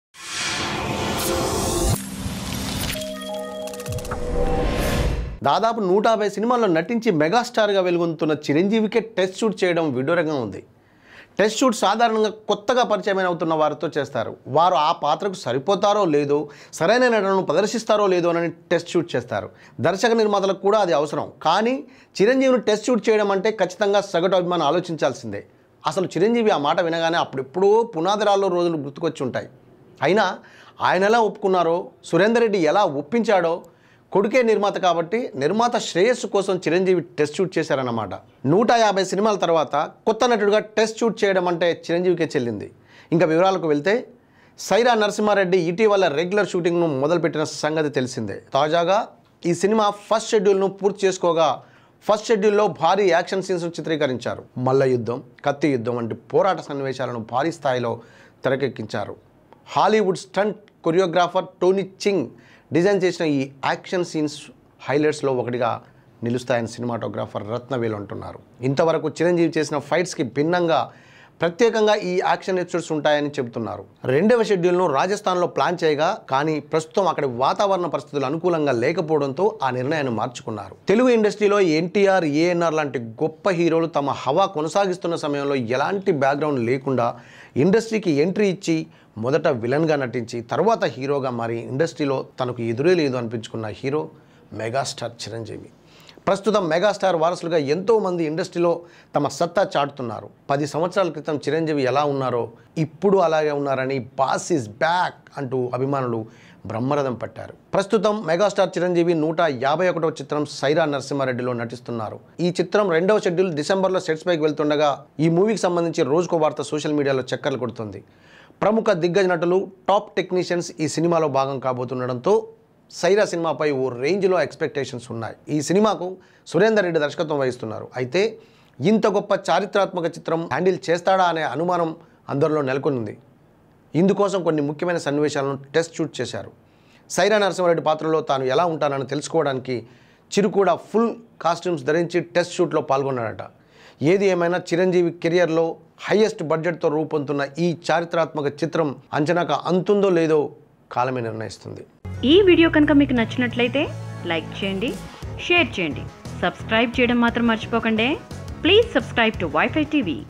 చిరంజీవిని టెస్ట్ షూట్ చేసిన డైరెక్టర్ Chiranjeevi || Y5 tv
చిరంజీవిని టెస్ట్ షూట్ చేసిన డైరెక్టర్ Chiranjeevi II y5tv II
Legendary actor chiranjeevi has been asked to test shoot for the film.